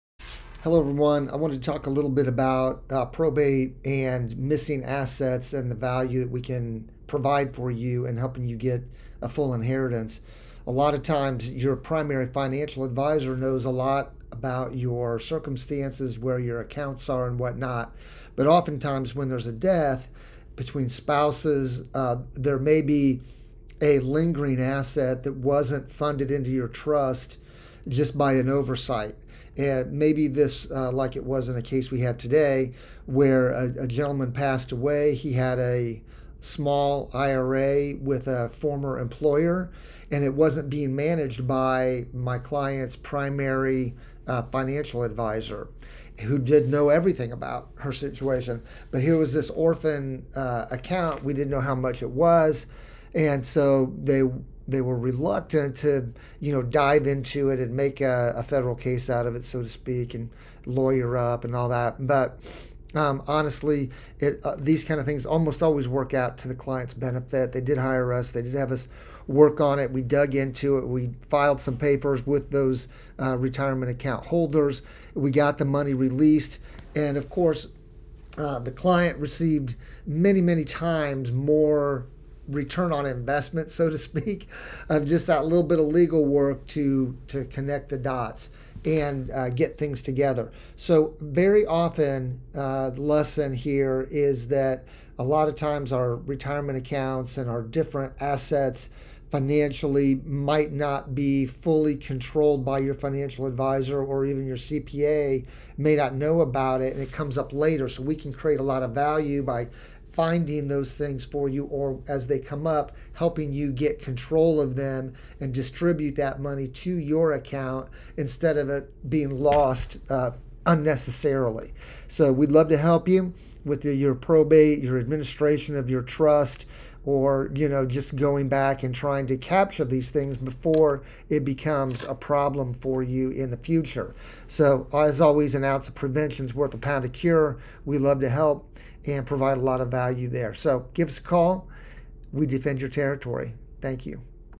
213 What about Missing Assets?????
Many times when a loved one passes there are unknown assets that are overlooked. Allow Jeff Jinks Law team help you with all your Estate Planning & Probate needs. We defend your territory & ensure you receive everything your passed loved one wanted. Click the link below to book your 30min complementary consultation call Today!